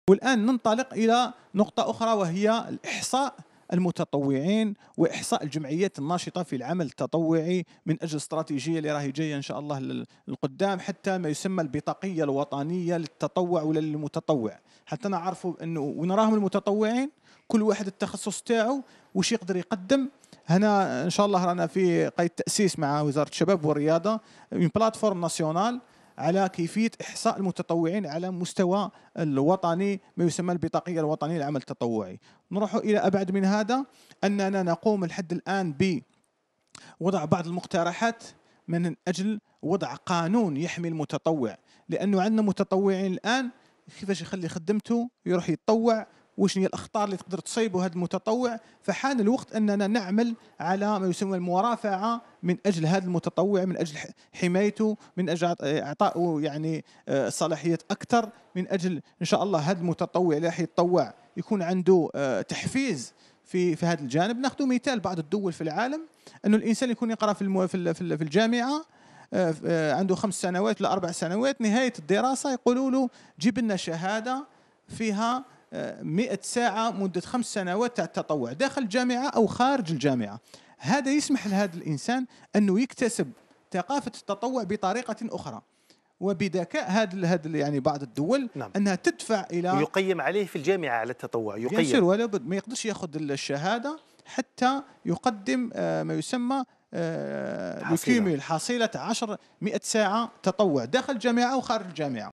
خالد مهيز..نسعى مع المسؤولين لوضع قانون يحمي المتطوع
HD 10992/5000
SD 10921/27500/V